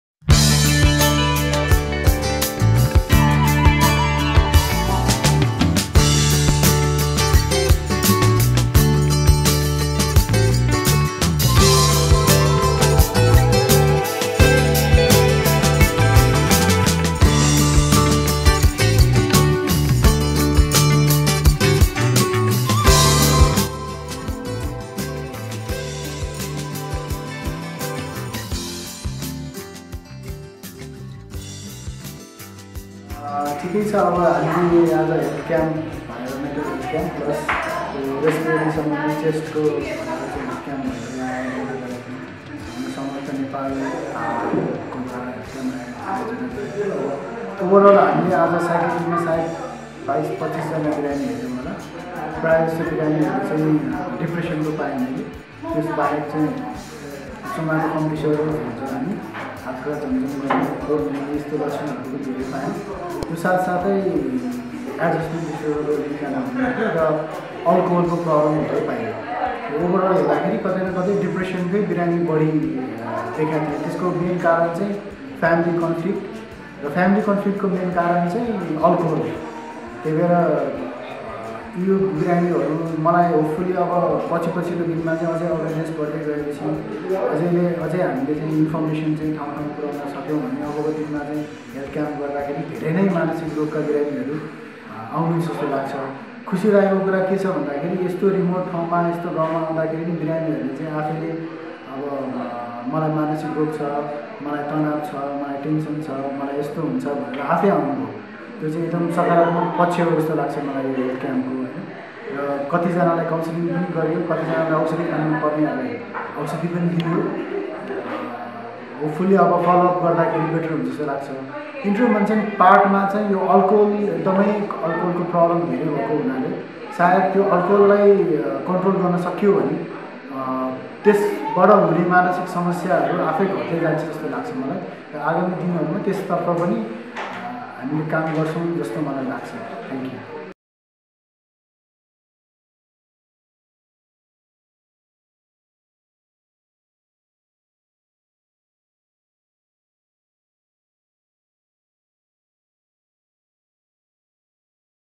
Janata Clinic | Psychological Awareness and Counseling Program at Dhokshan | Dr. Ritesh Thapa
A psychosocial screening and basic counselling program was conducted, under Samartha Dhokshan program of Samartha Nepal, in supervision of Consultant Neuropsychiatrist Dr. Ritesh Thapa , Director of Rhythm Neuropsychiatry Hospital And Research Center Pvt. Ltd.

Among the 60 residents of the Tamang community, who participated in the program, 22 opted for psychiatric counselling - 8 were male and 14 female. A few were diagnosed as requiring medication and one was referred for hospitalized management. The major problems observed in this community was depression due to early marriage, family issues and alcohol dependence.

Counselling psychologist Rabina J. Shresthahrestha, from Koshish Nepal and Mamata ShresthaShrestha from Trichandra College also accompanied us in the program.

We would like to thank all those , who were involved directly or indirectly in the program, for their support in making the program a success.